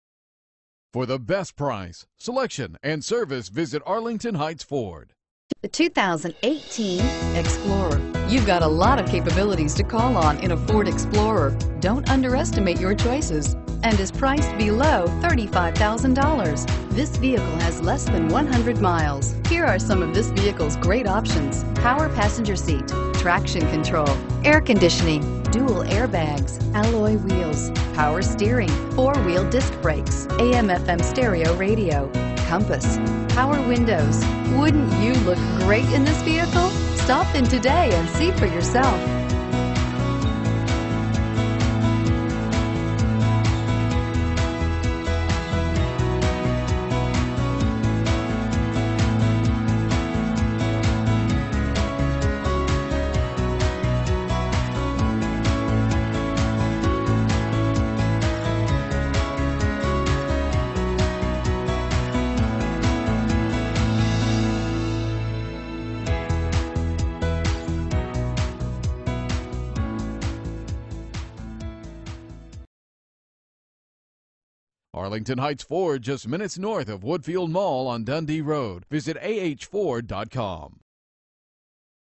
2018 Ford Explorer Arlington Heights IL 0S182535
2018 Ford Explorer XLT

801 Dundee Rd.

Turbocharged! All the right ingredients! If you demand the best, this fantastic 2018 Ford Explorer is the SUV for you. It is nicely equipped. This Explorer will take you where you need to go every time...all you have to do is steer! Price includes: $250 - Retail Customer Cash. Exp. 11/30/2017, $500 - Ford Credit Retail Bonus Customer Cash. Exp. 11/30/2017, $750 - Retail Bonus Customer Cash. Exp. 11/30/2017